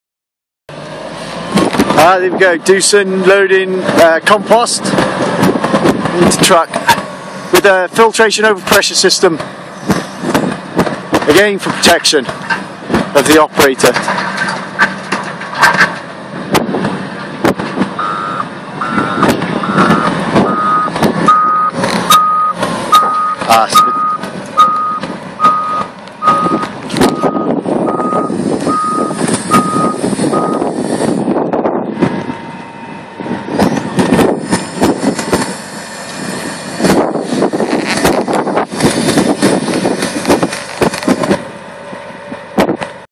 Doosan loading shovel
Doosan loading shovel fitted with Arctic Air filtration over pressure system working in the composting industry.
protection against dusts and gases.